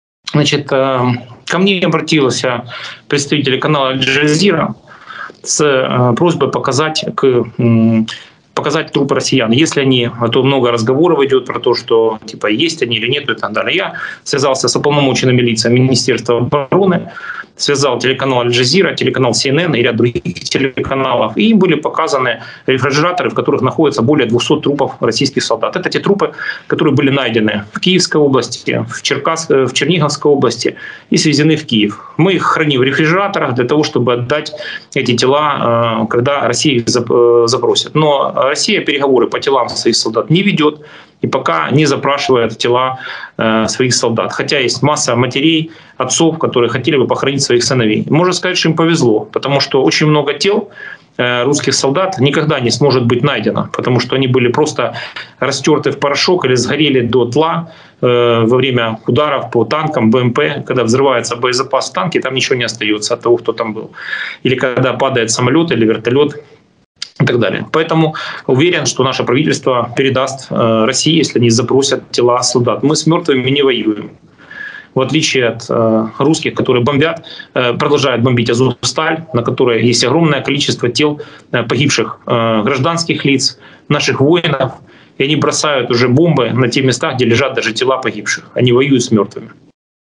Более 200 тел солдат рф хранятся в рефрижераторах, но россия не ведет переговоров, чтобы их забрать.
Интервью Дмитрия Гордона с советником министра внутренних дел Украины Антоном Геращенко. 2022 год.

І